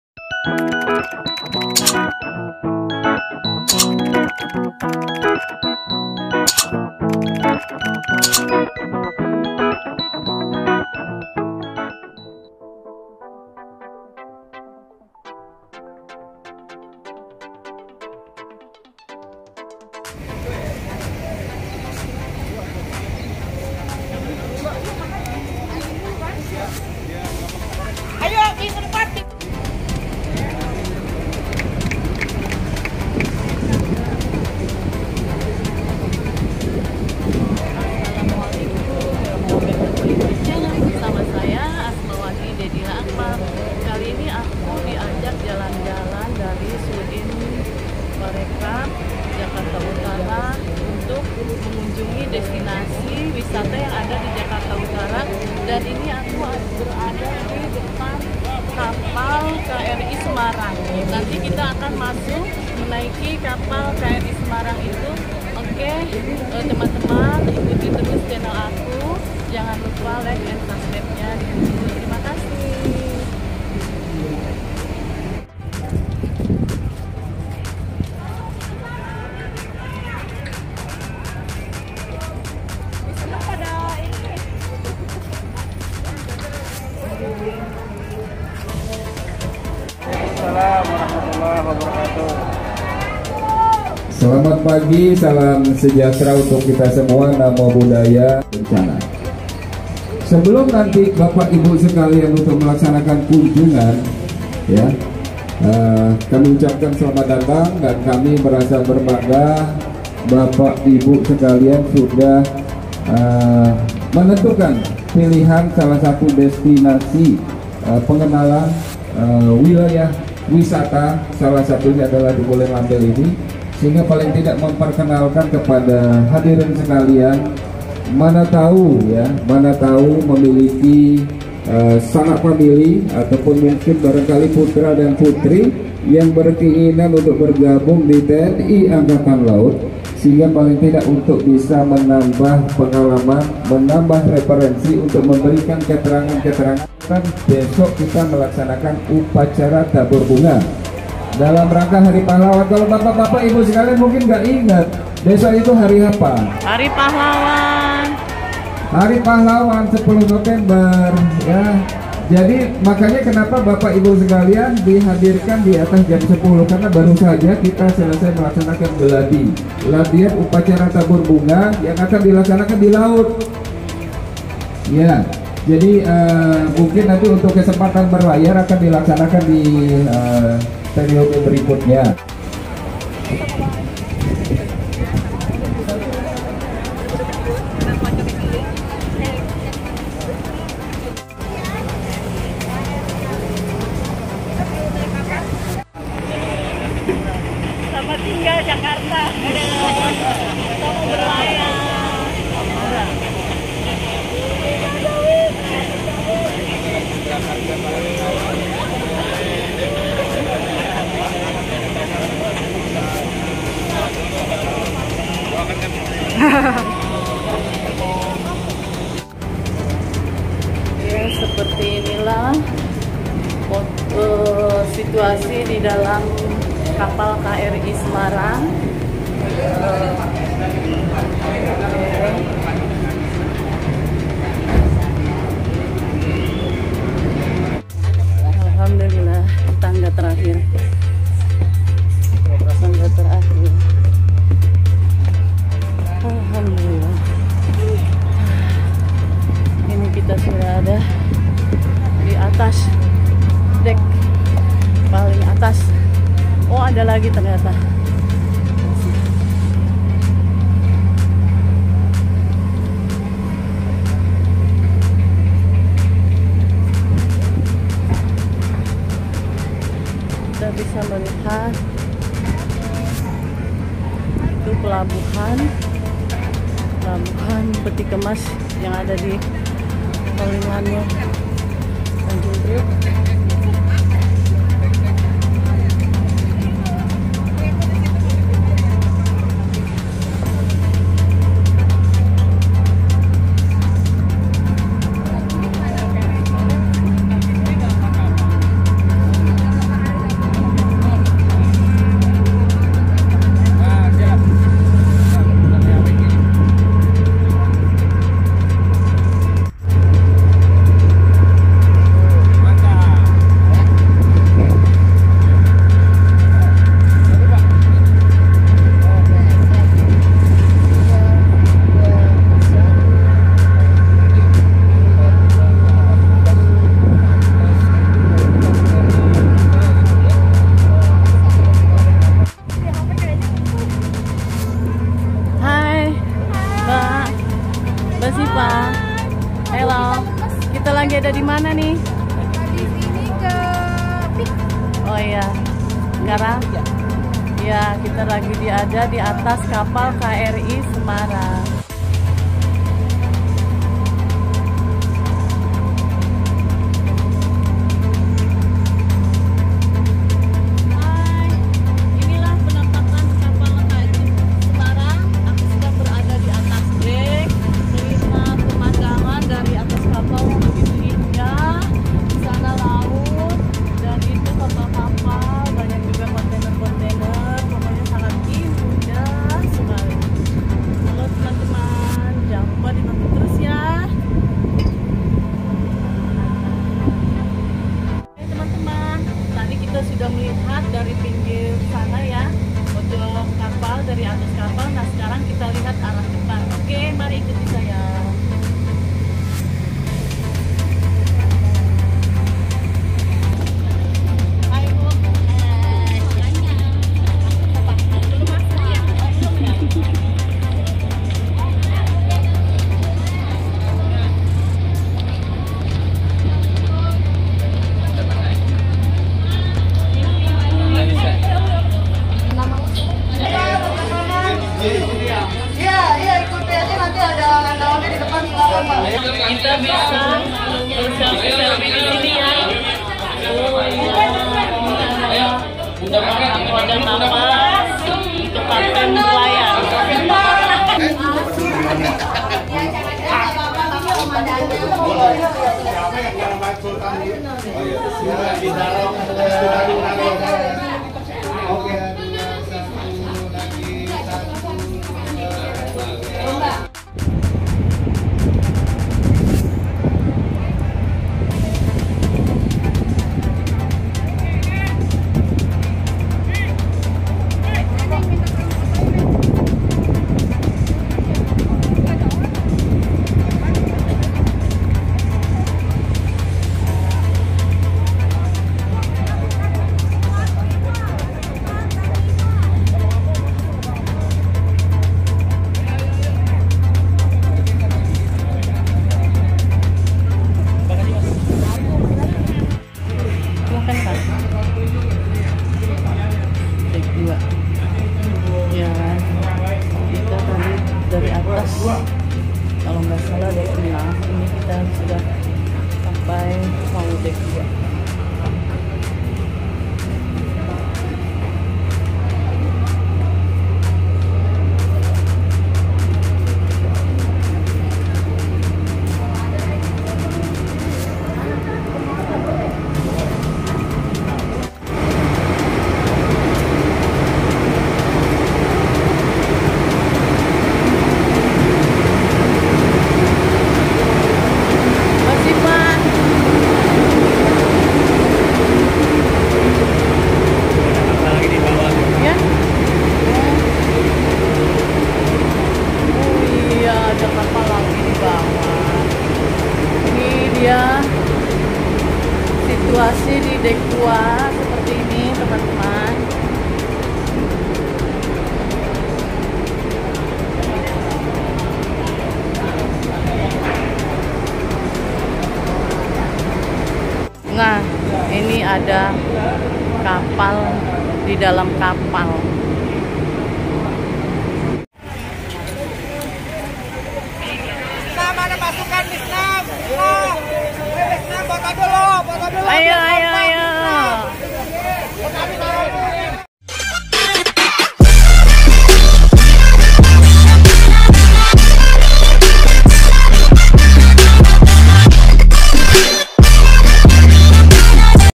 BERKUNJUNG KE DESTINASI KRI SEMARANG II KOLINLAMIL TG PRIOK
Destinasi KRI Semarang KOLIN LAMIL TG PRIOK

Alhamdulillah wasilah dari Sudin Parenkraf Jakarta Utara saya bisa masuk mengunjungi Destinasi wisata yang ada di Jakarta Utara ,salah satunya KOLINLAMIL Tg Priok untuk melihat dan masuk ke dalam kapal laut KRI Semarang yang sedang bersandar